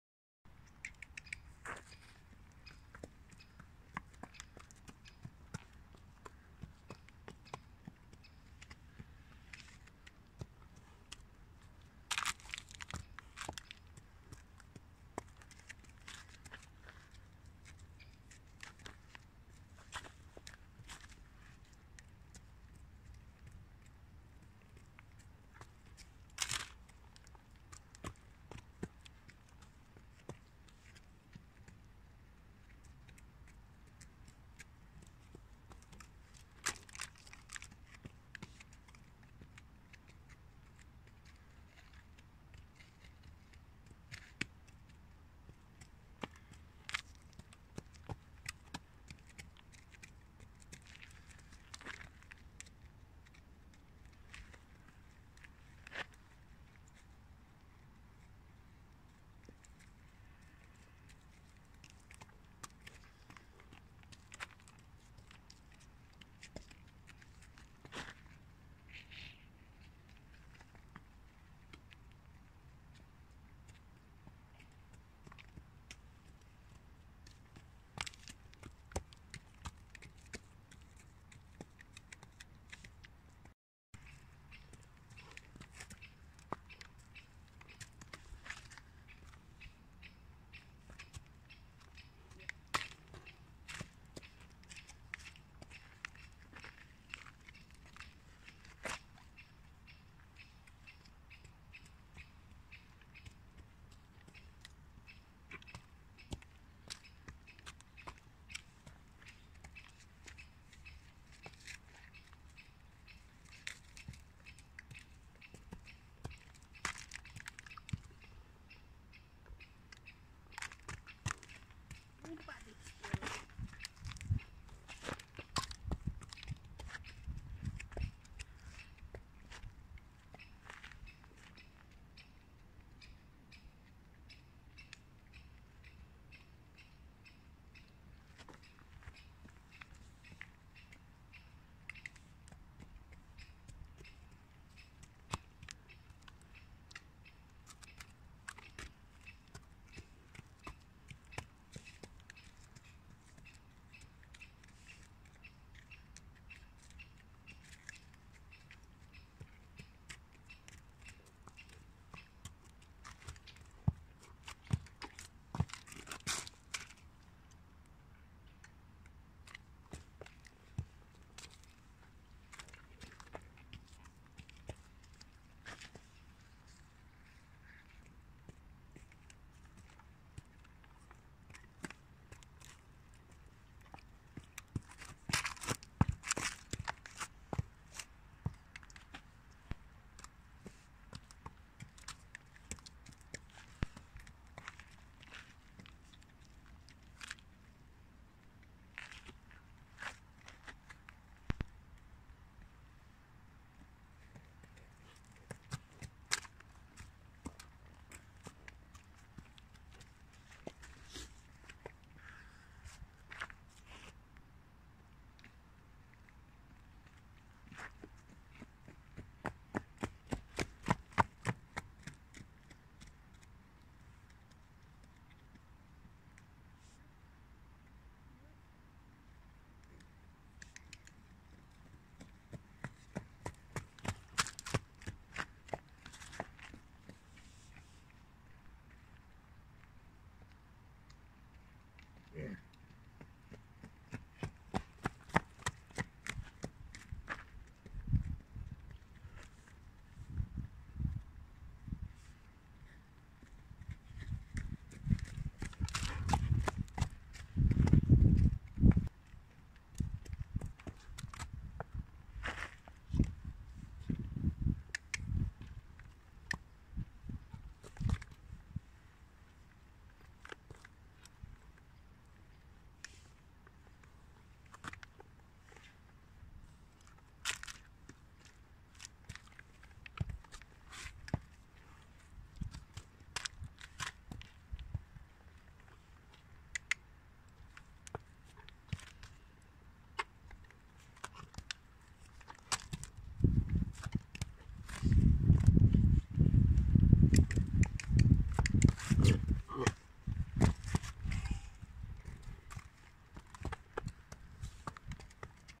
Horse crushing chocolate candies
Horse is walking over and crushing some candies, snickers and choco pie